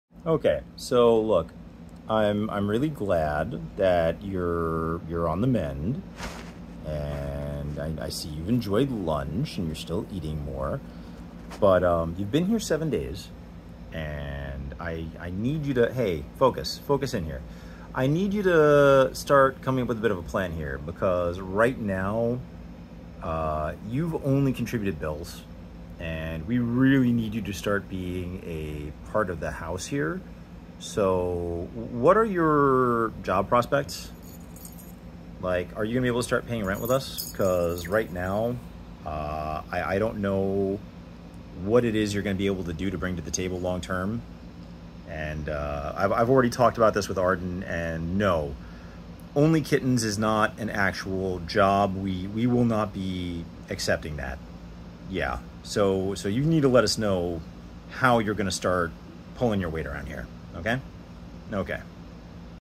Rescue Kitten Day 7 - Have they already been mooching off us for a week?
Asking the kitten the tough questions about their long term ambitions and how they will meet their financial responsibilities to the home.